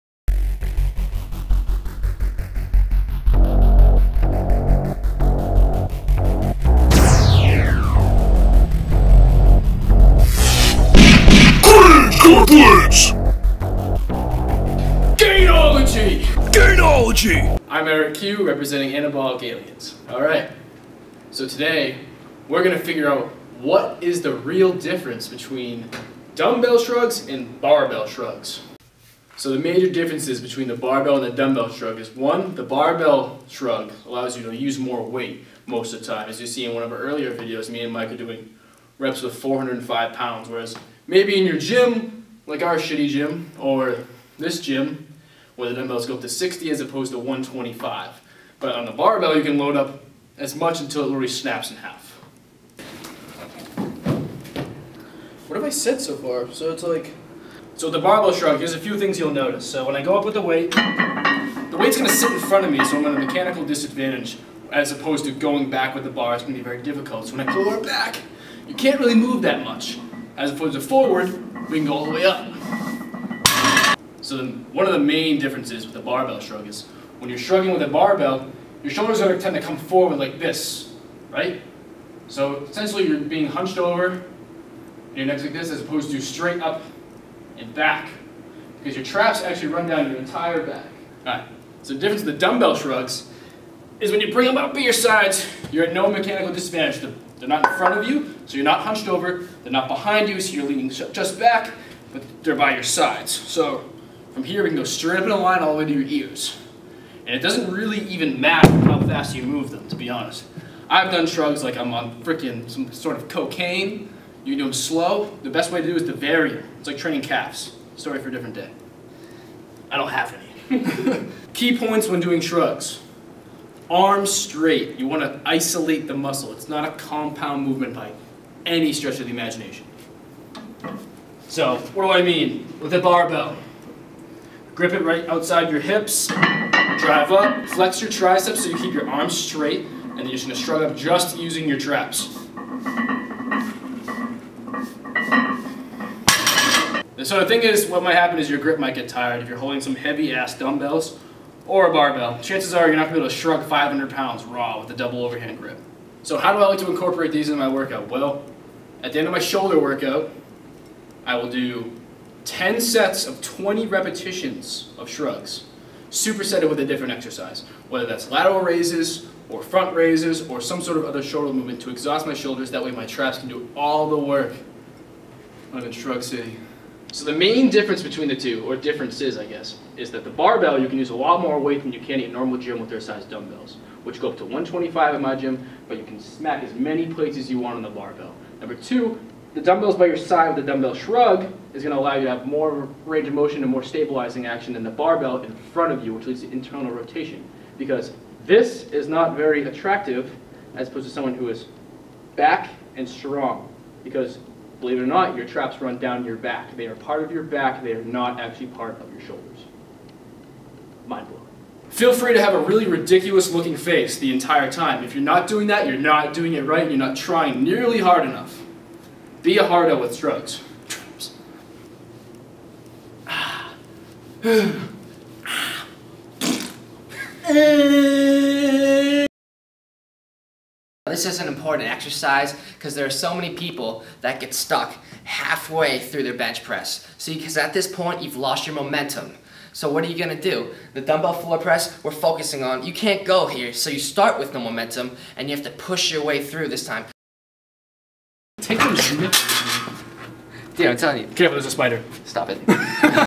Barbell VS Dumbbell Shrugs
Gainology:

So what are the differences between BARBELL shrugs and DUMBBELL shrugs? Well watch this video to find out, as we cover the many differences between the two. TRAPS!